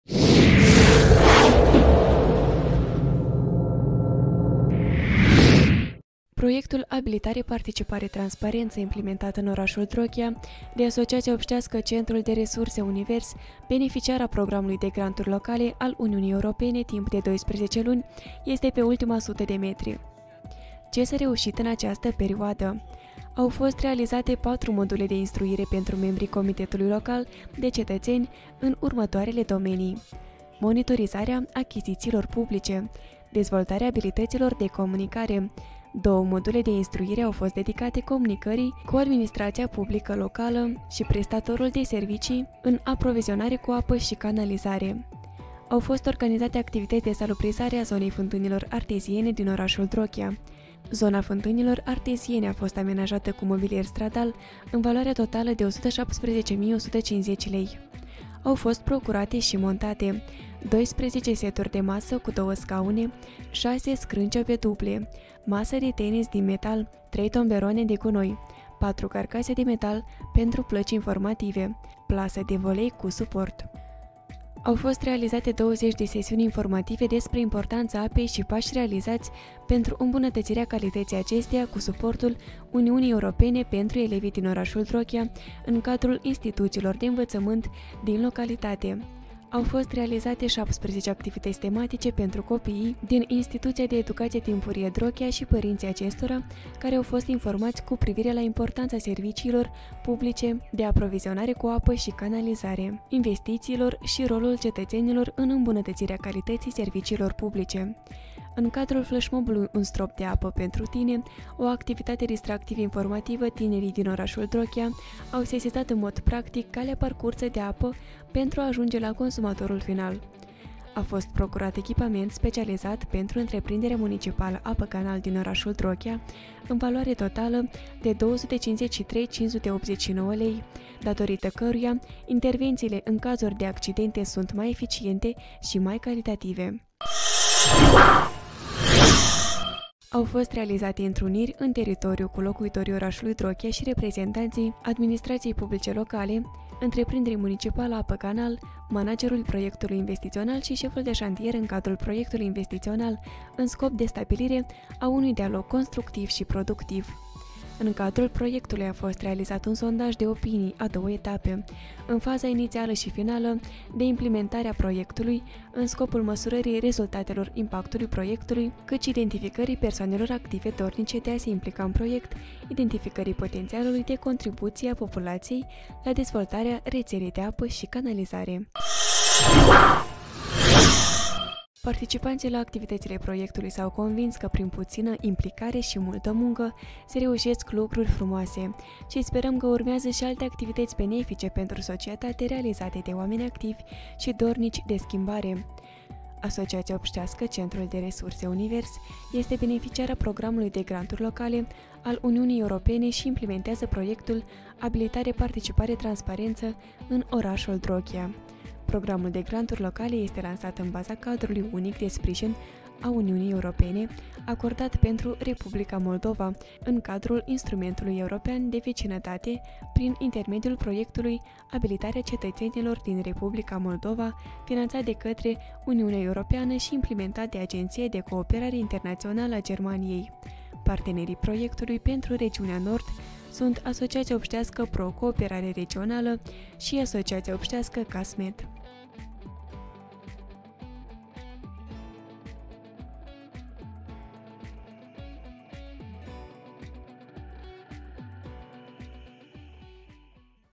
Proiectul Abilitare, Participare, Transparență pe ultima sută de metri / Drochia în obiectiv
Proiectul APT implementat în orașul Drochia de  AO ”Centrul de resurse Univers”, beneficiară a Programului de granturi locale al Uniunii Europene , timp de 12 luni este pe ultima sută de metri.